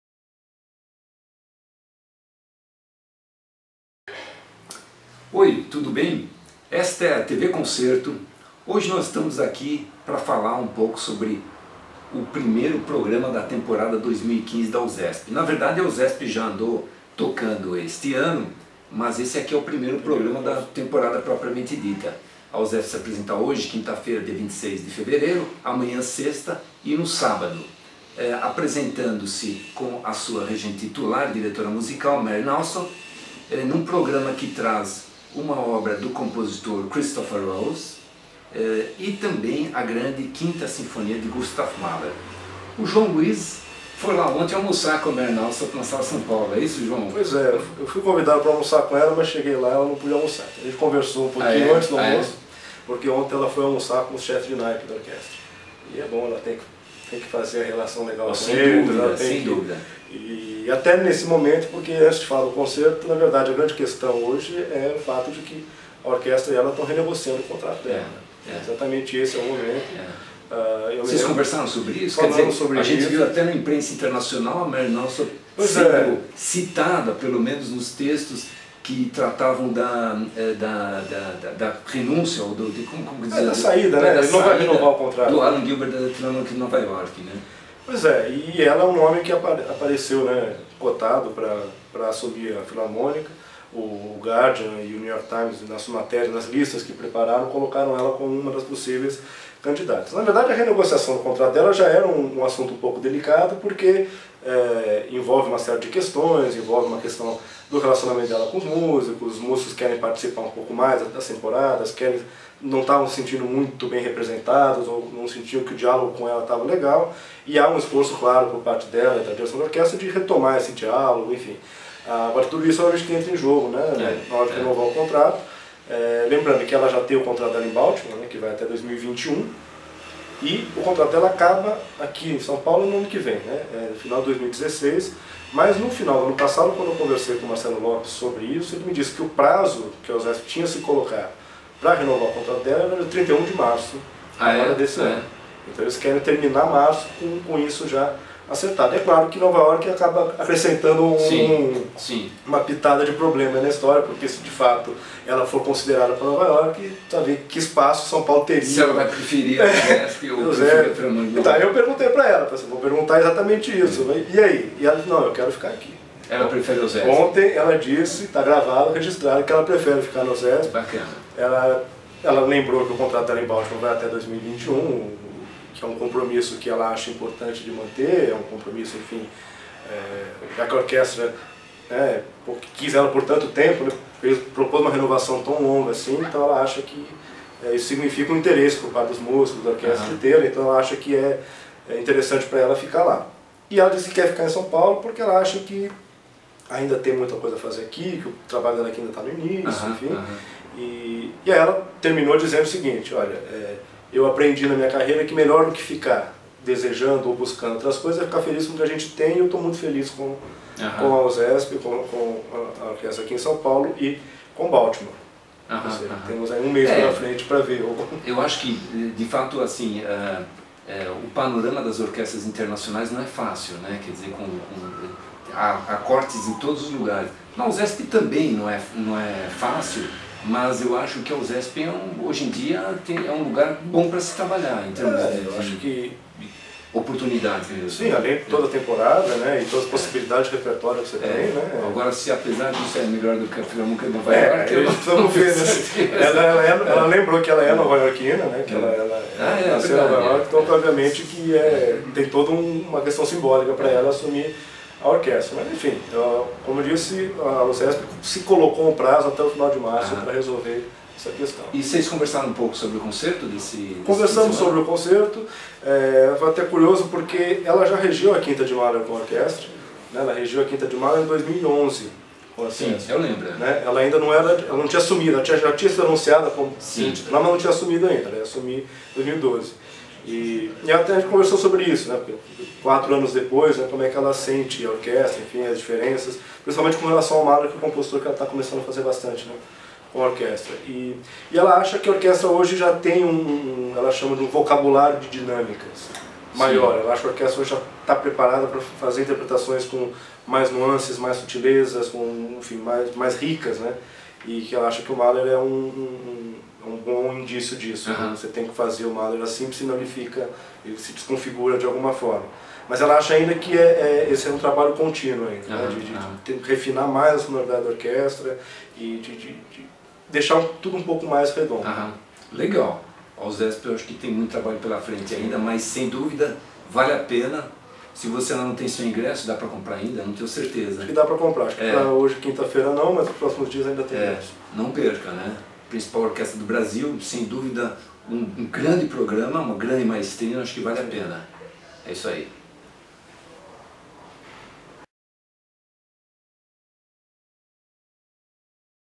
tvCONCERTO: Marin Alsop e Osesp (fev 2015)
Nelson Rubens Kunze conversa com João Luiz Sampaio, que se encontrou com a maestrina Marin Alsop para falar sobre a renovação de seu contrato e sobre o primeiro programa da temporada da Osesp. [Gravação realizada em 26 de fevereiro de 2015.]